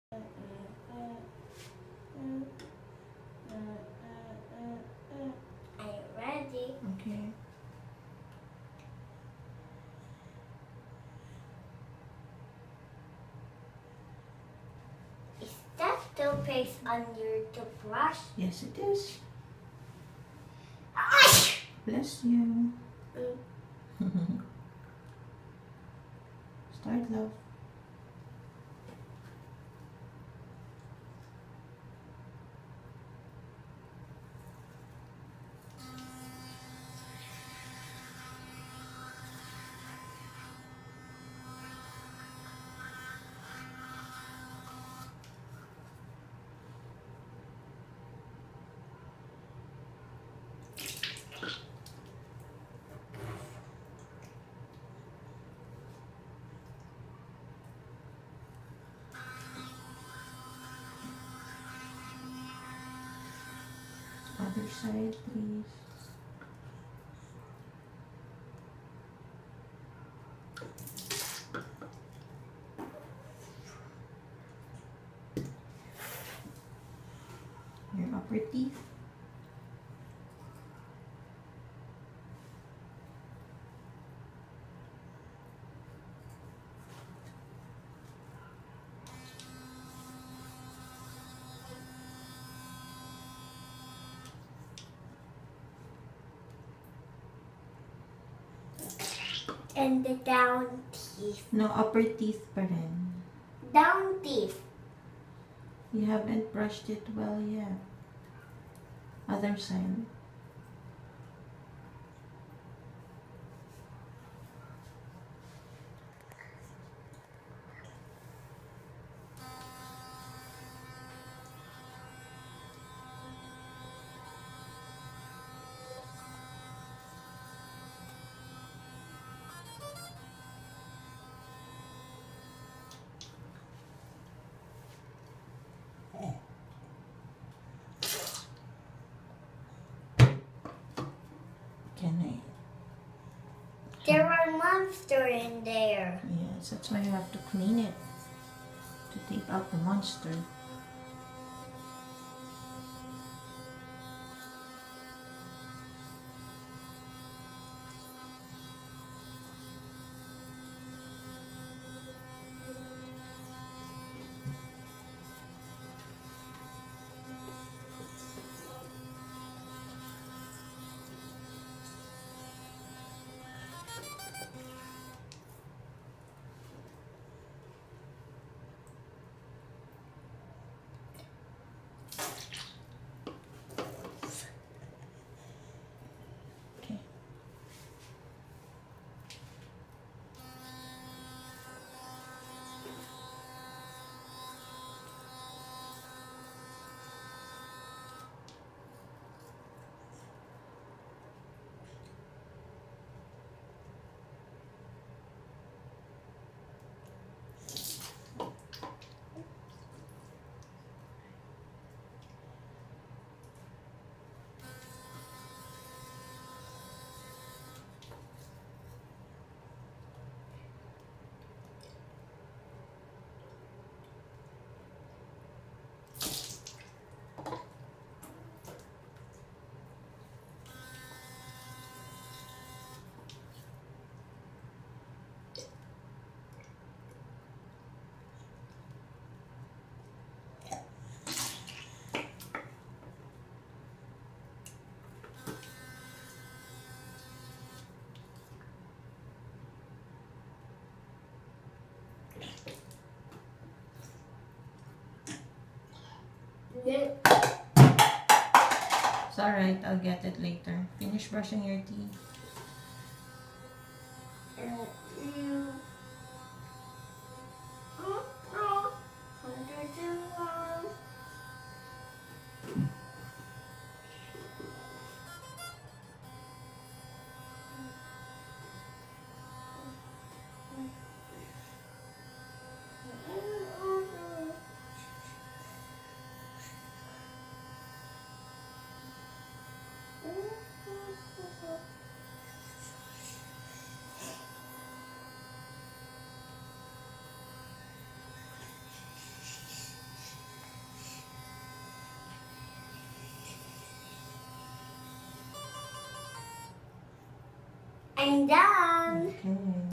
Brush Teeth
Sinag brushing teeth at 4 years old ❤️

***he uses filtered water for brushing